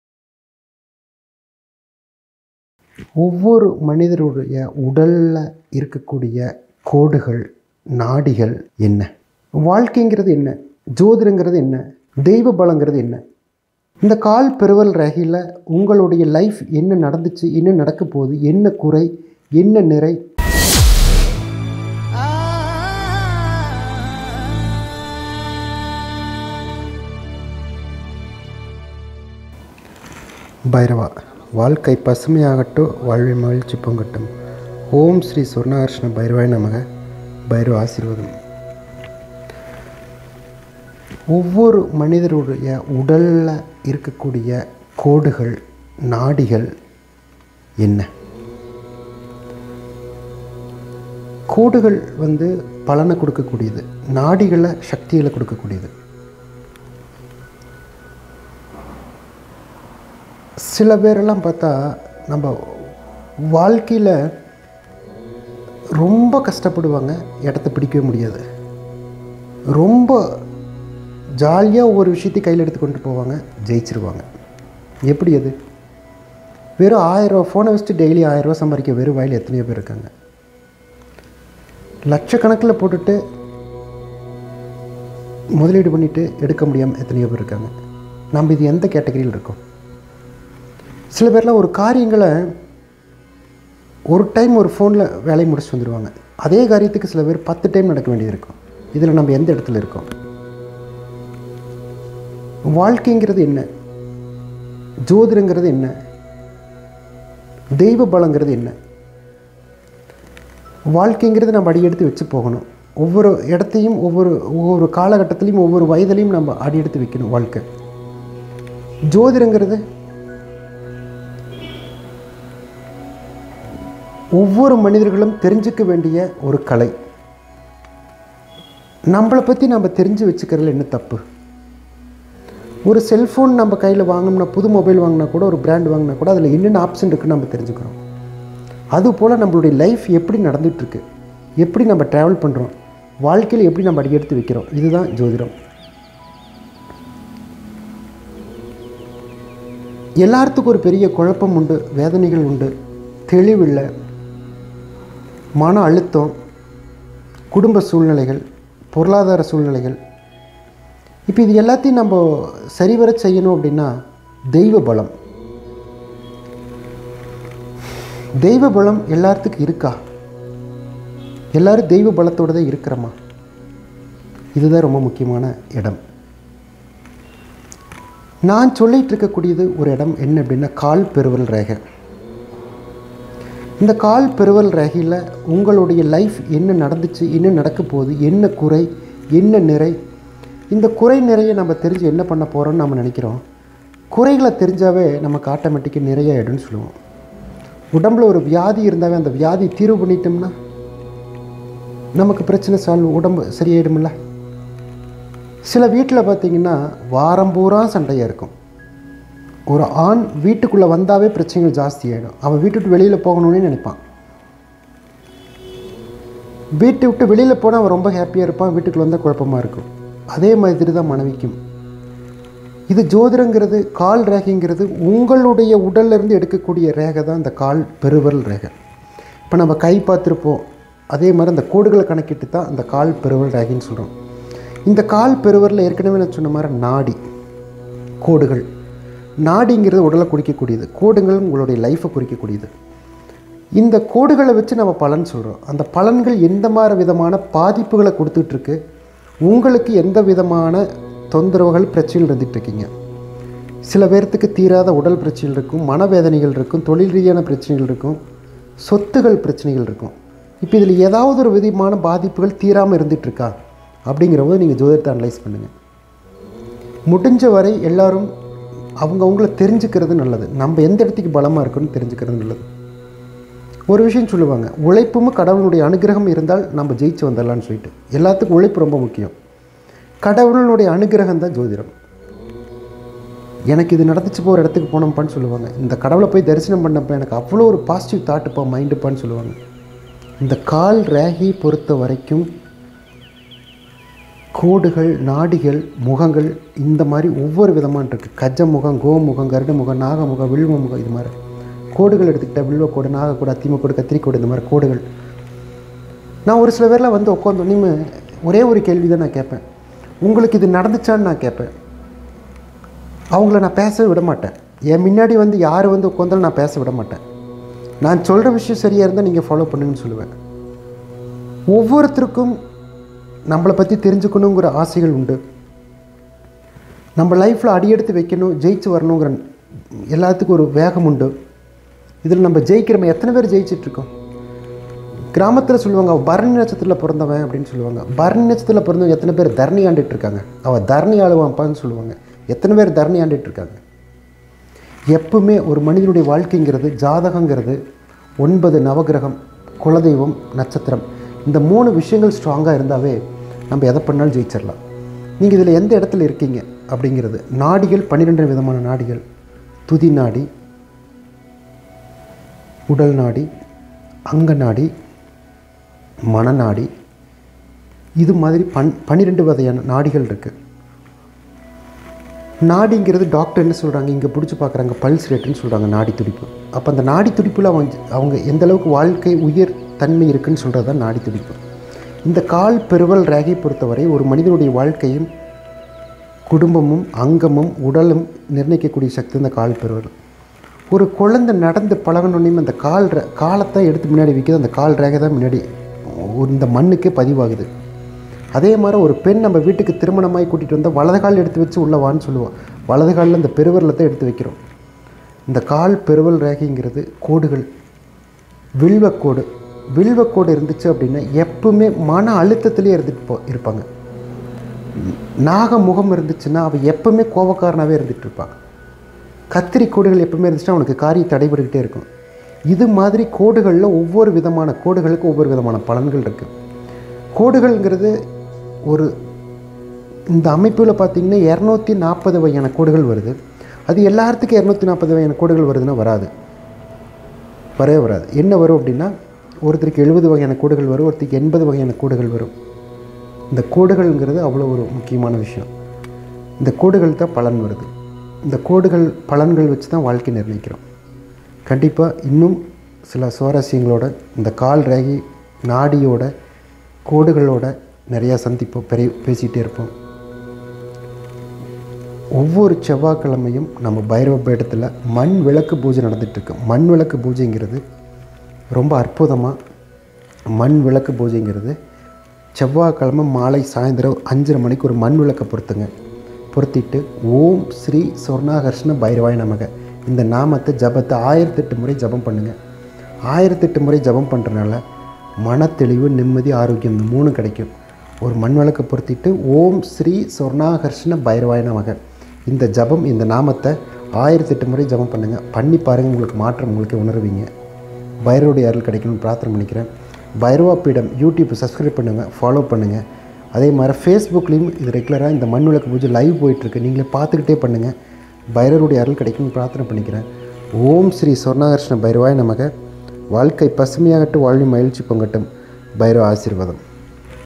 மனித உடம்பில் உள்ள கோடுகள் மற்றும் நாடிகளின் பலன்கள் என்ன?
✅பாத ரேகை கோடுகளுக்கும், நாடிக்கும் உள்ள வித்தியாசம்.
✅கோடுகளால் ஏற்படக்கூடிய  நன்மைகள் மற்றும் நாடியினால் பெறக்கூடிய சக்திகள்.
✅வாழ்வில் ஏற்படக்கூடிய துர்பலன்கள்  அனைத்தையும் நற்பலன்களாக மாற்றுவதற்கான வழி.
✅எடுத்த செயலில் வெற்றி பெற, வாழ்வில் முன்னேற்றம் பெற உதவும் பாத கோடுகள்.